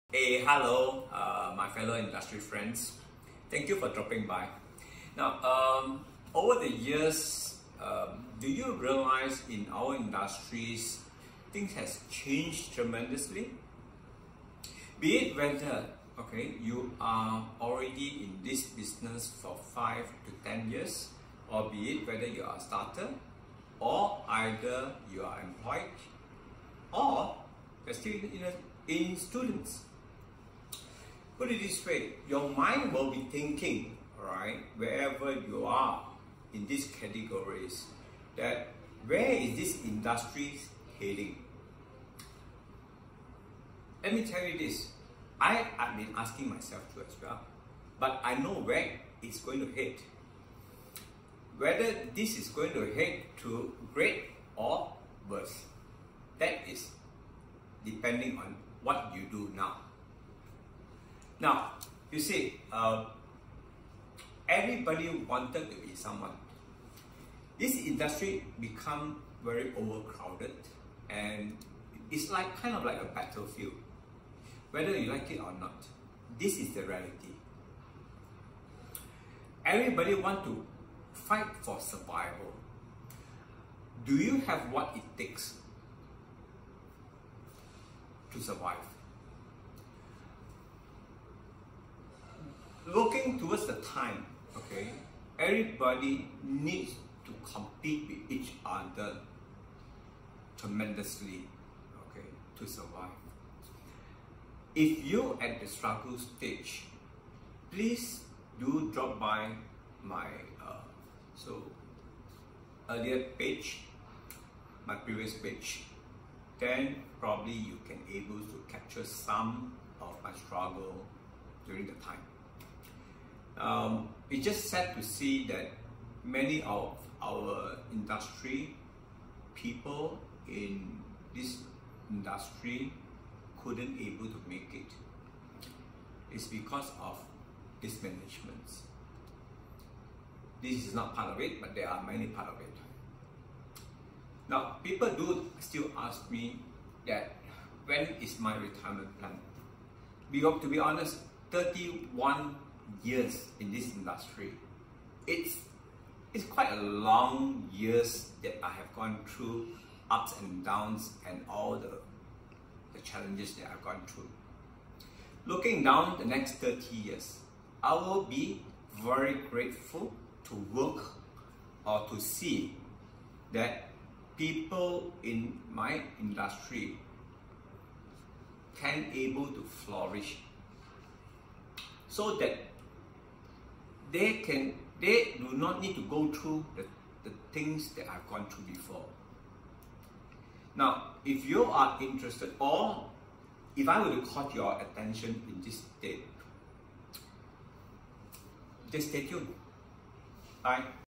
To 30 years and beyond - The Interior Design Business
“He says 30 years of running his interior business is not as important at his outlook into his next 30 years of his passion craft!”
"室内设计的业务 ”比起经营室内设计业务30年的自己，他更重视未来30年对于这份行业的激情。